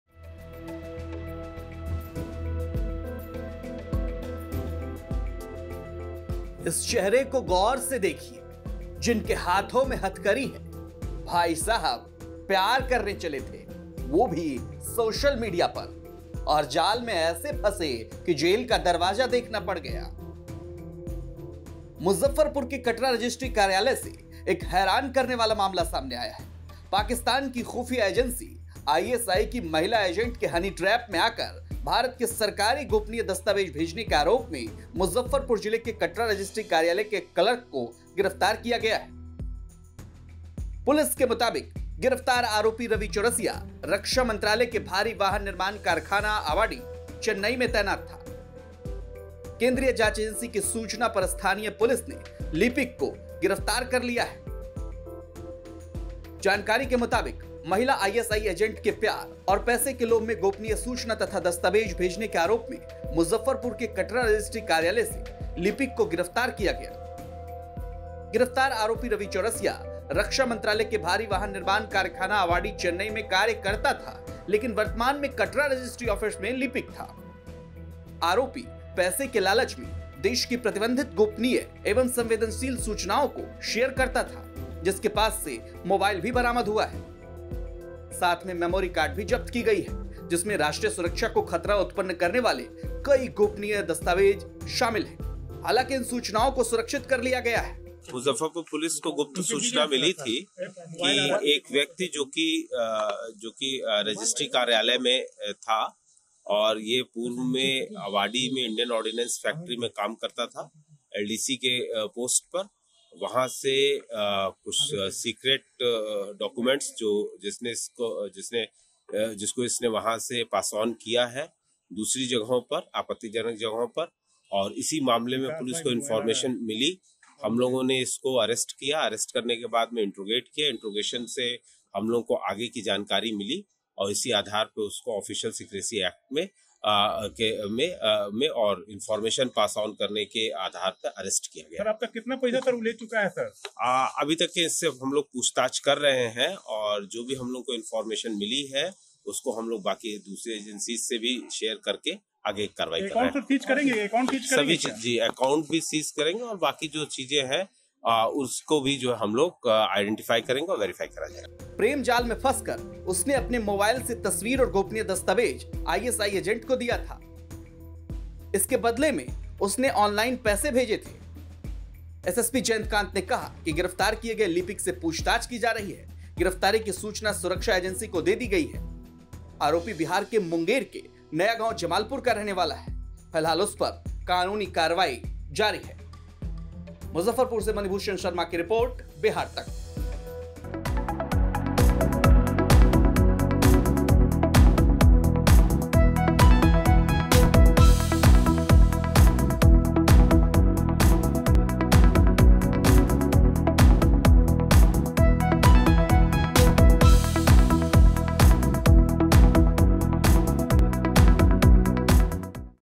ISI की 'हसीना' के हनीट्रैप में फंसा क्लर्क, प्यार में पागल होकर शेयर करता था खुफिया सूचना| Bihar Tak
महिला isi एजेंट के प्यार में और पैसे के लालच में रक्षा मंत्रालय की गोपनीय दस्तावेज देने और पैसा लेने के आरोप में मुजफ्फपुर से एक लीपिक को गिरफ्तार किया गया है।